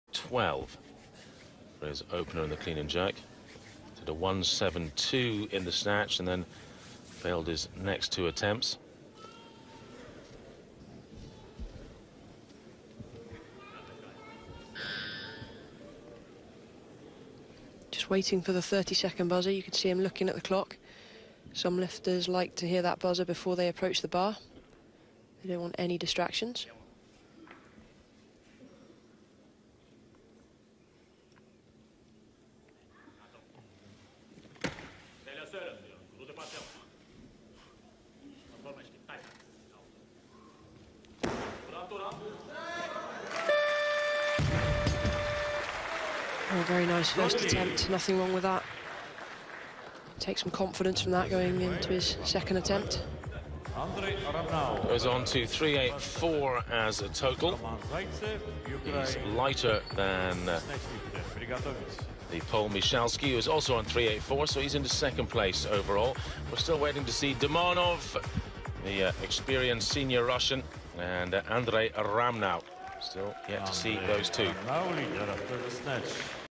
PLESNIEKS Arturs 1j 212 ewc 2014 cat. 105
European Weightlifting Championship 2014 Tel Aviv,Чемпионат Европы по тяжёлой атлетике 2014 прошёл в Тель-Авиве